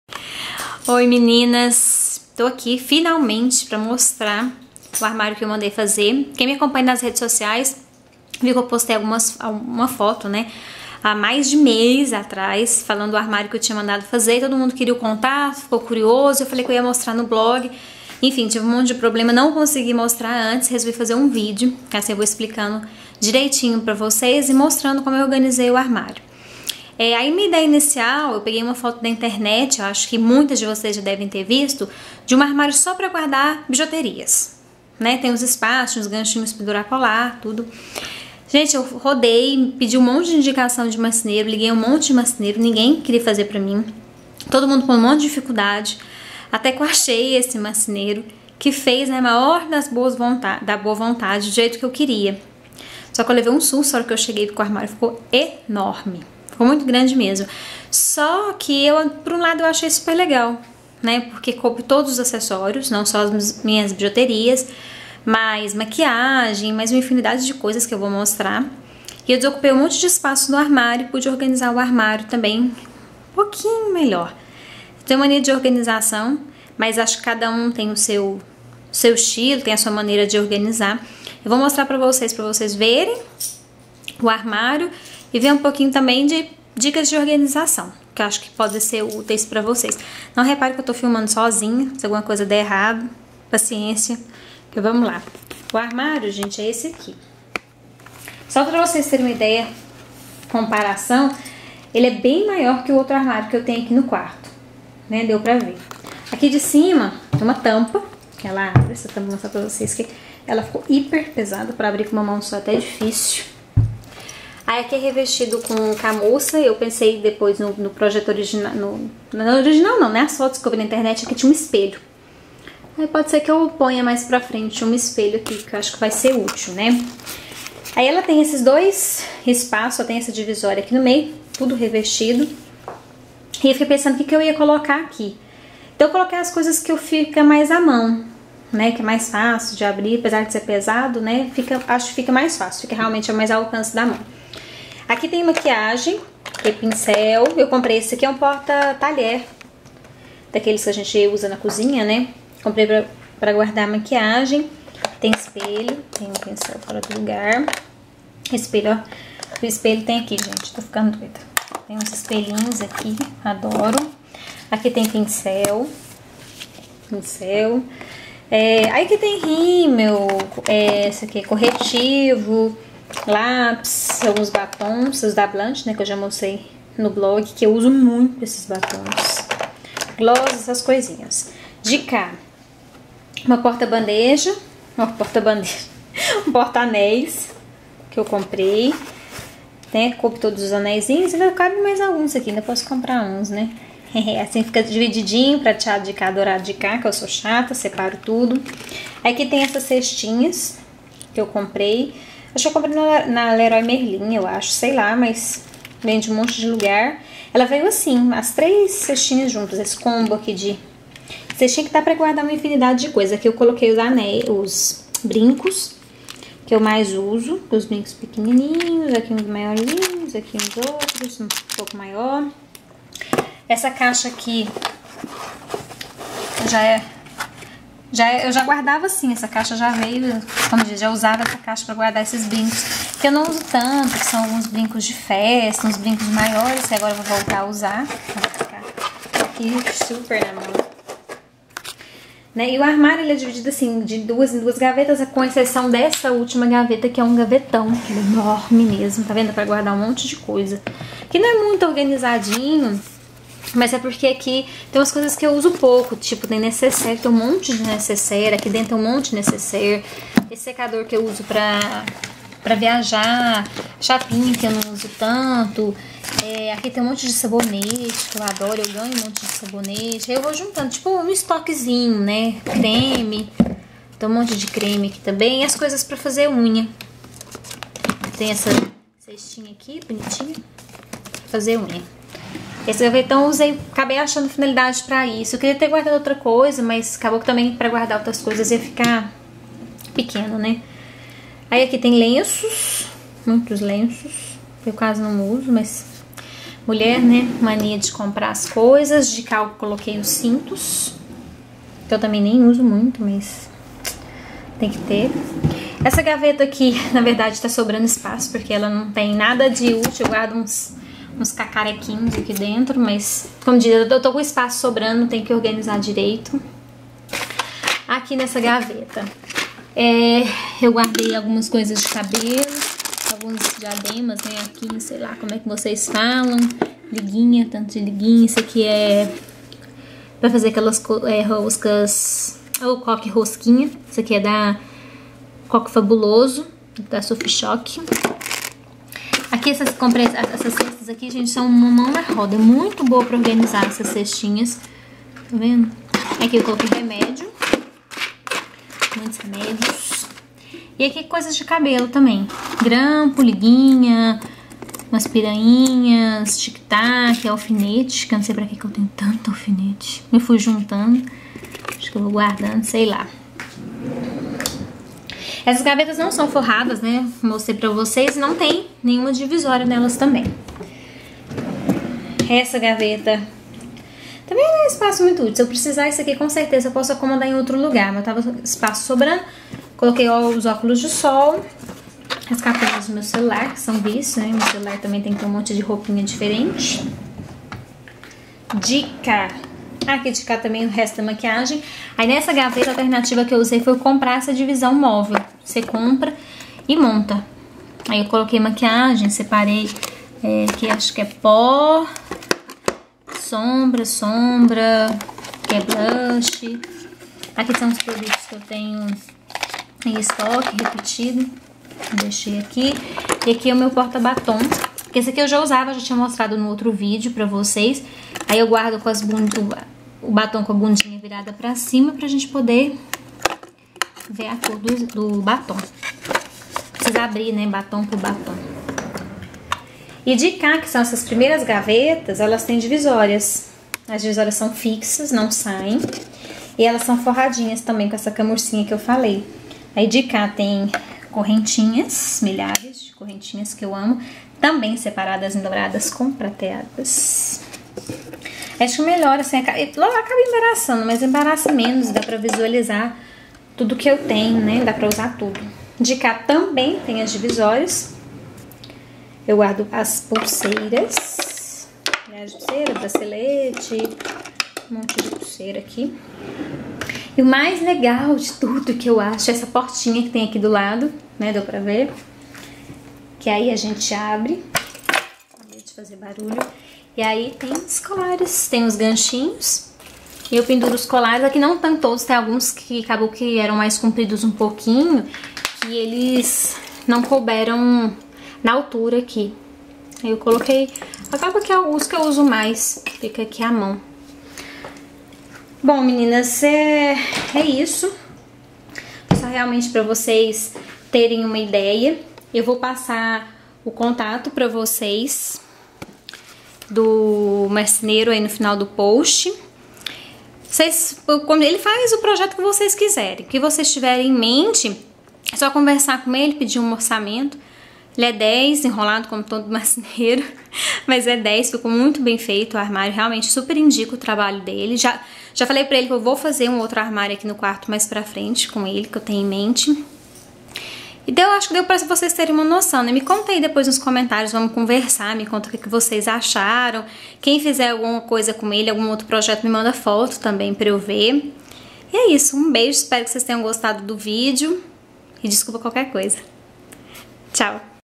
Armário para acessórios
Armário que eu mandei fazer para guardar meus acessórios.